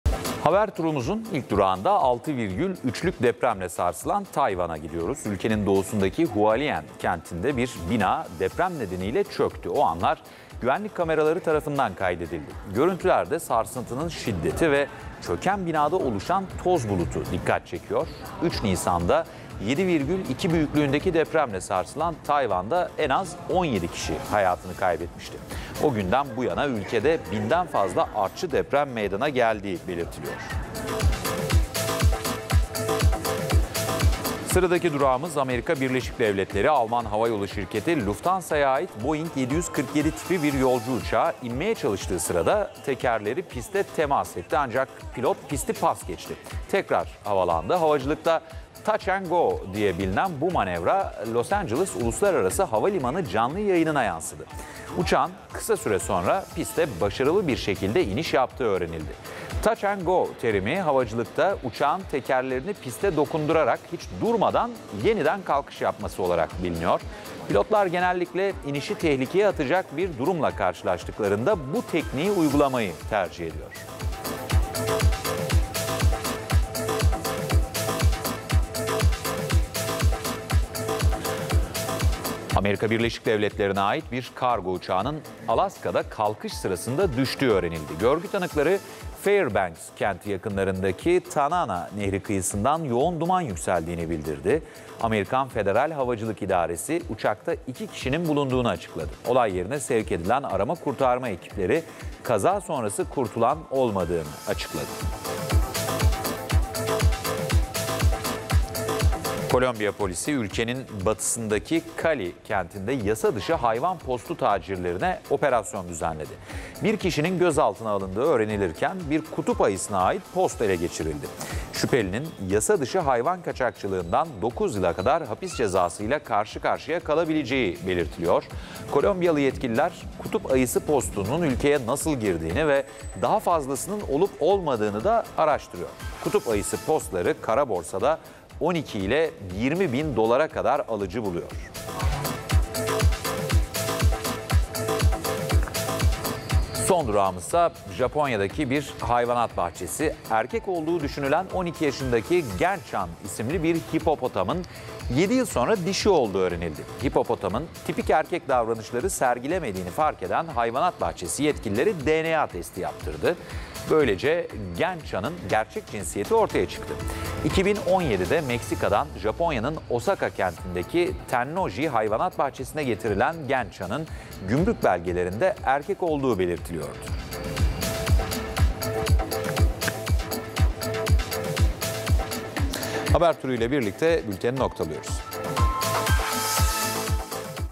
Tayvan'da Meydana Gelen Depremde Bina İşte Böyle Çöktü! | NTV
Tayvan'daki Depremde Bina Çöktü

Tayvan'ın doğusundaki Hualien kentindeki bir bina, deprem nedeniyle çöktü. O anlar güvenlik kameraları tarafından kaydedildi. Görüntülerde sarsıntının şiddeti ve çöken binada oluşan toz bulutu dikkat çekiyor. 3 Nisan'da 7.2 büyüklüğündeki depremle sarsılan Tayvan'da, en az 17 kişi hayatını kaybetmişti. O günden bu yana ülkede binden fazla artçı deprem meydana geldiği belirtiliyor.